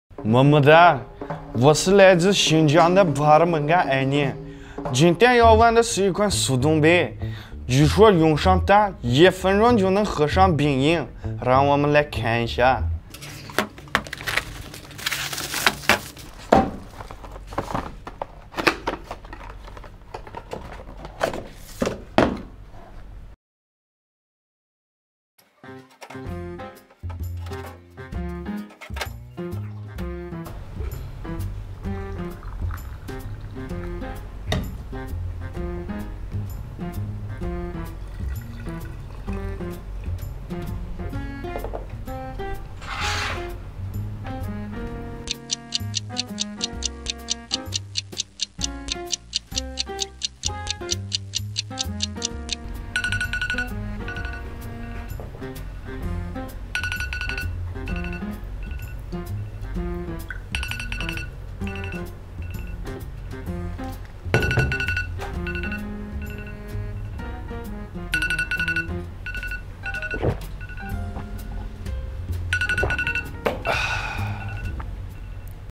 【卡奇手大開箱】24比55°杯還強的黑科技速凍杯，能讓妳壹分鐘喝上冰飲
關注Wechat公眾號:卡奇手，微博:卡奇手更多有趣的事等著各位。